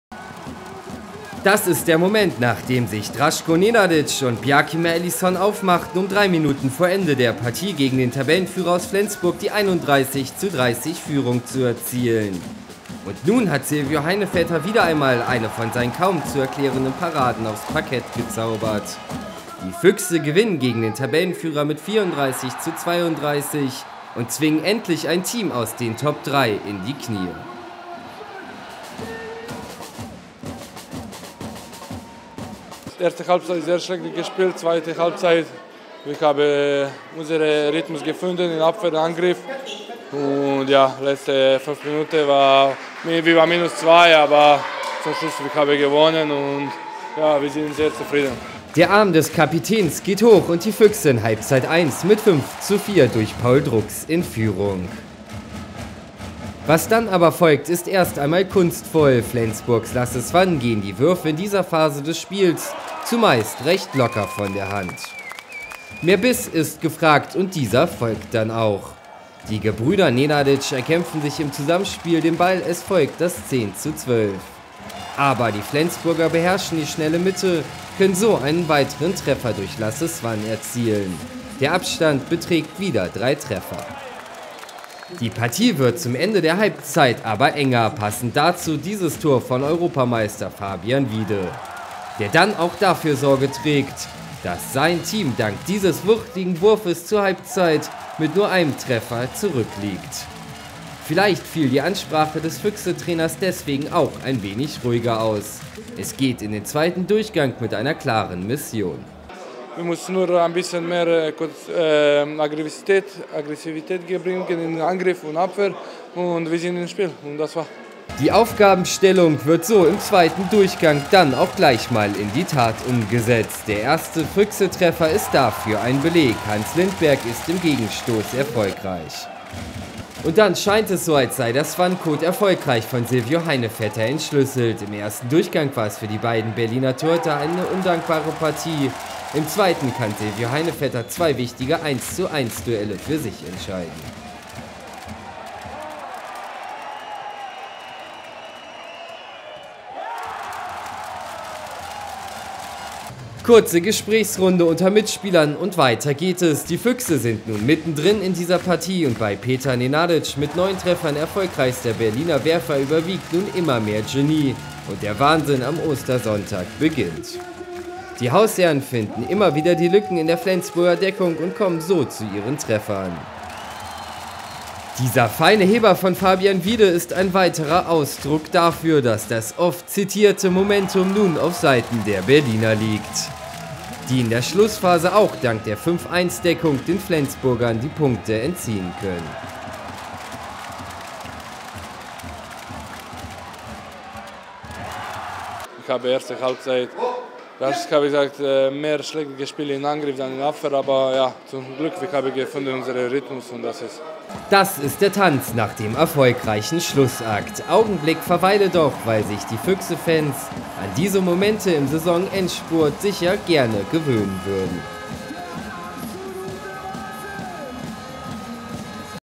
Füchse Berlin schlagen Flensburg
Füchse schlagen Tabellenführer Flensburg

Die Füchse Berlin haben die SG Flensburg-Handewitt mit 34:32 (16:17) besiegt. Vor 9.000 Zuschauern erwischten die Gäste aus Flensburg den besseren Start und lagen über weite Strecken der ersten Halbzeit vorn. Doch die Berliner drehten in den letzten fünf Spielminuten noch einmal auf und konnten vor allem auf Silvio Heinevetter und Petar Nenadic bauen. Die besten Werfer der Füchse waren Petar Nenadic mit neun und Hans Lindberg mit acht Treffern.